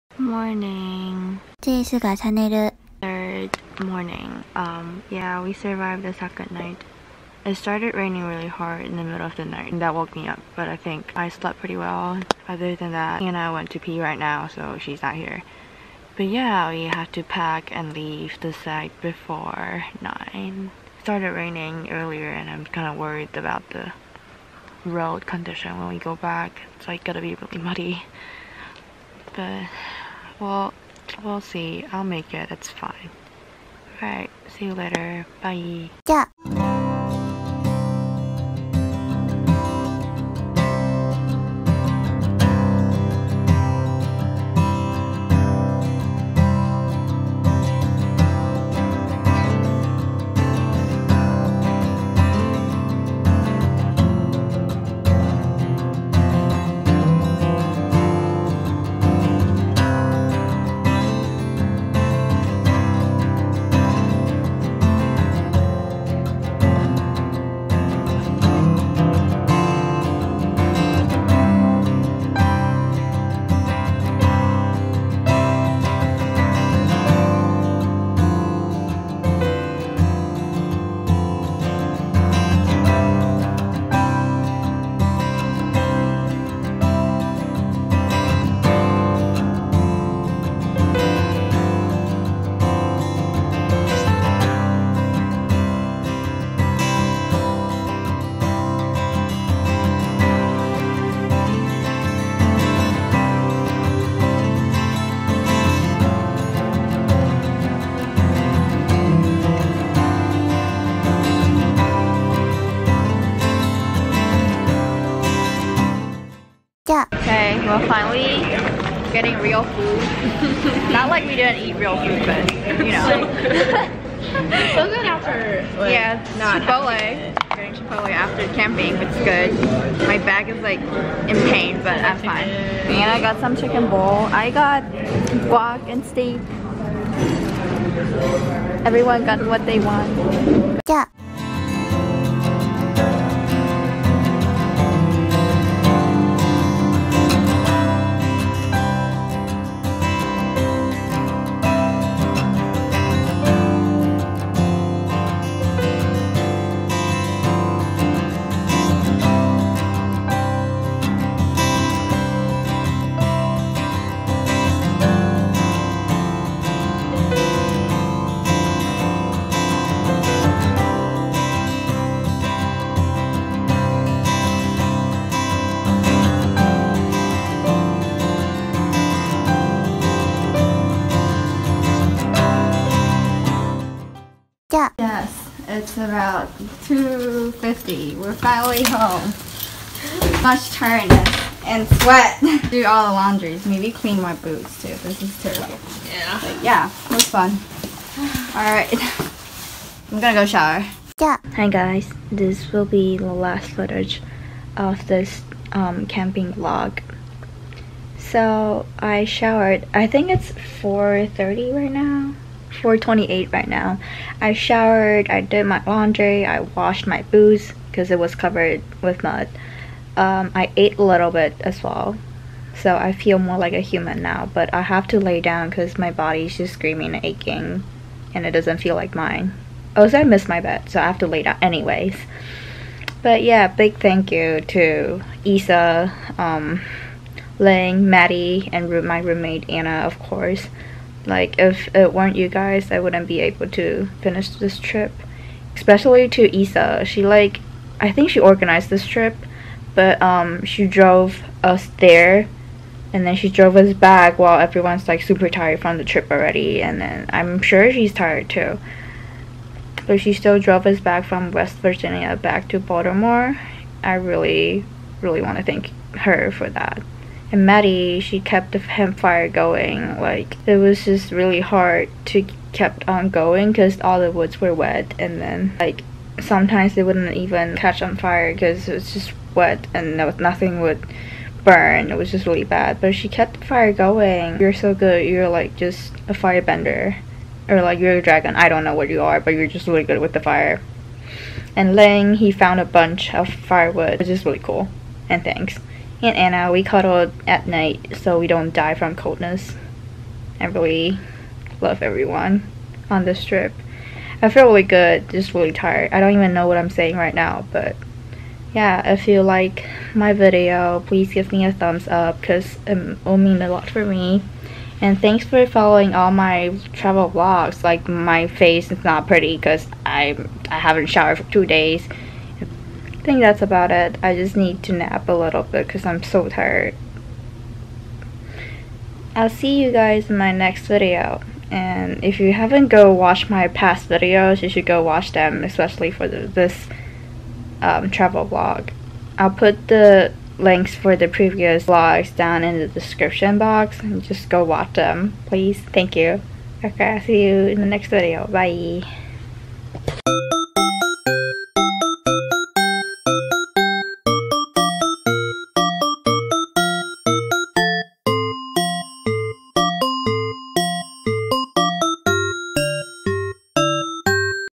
【Travel】Bear Rocks Trail Camping Trip Day3 | ENG Vlog
Continue from the previous vlog for packing, day 1, and day 2 of the trip.

We went on a 2 nights camping trip to Bear Rocks Trail, WV for our school's fall break.
This trip was pretty intense for me since I haven't been on a trail or go camping for a long time.

We made it back to Baltimore! Everyone's safe and happy!
Living without clean water, electricity and phone service for 3 days was really intense, but I did have a lot fun.
I won't ever forget this special experience with these lovely human beings 😘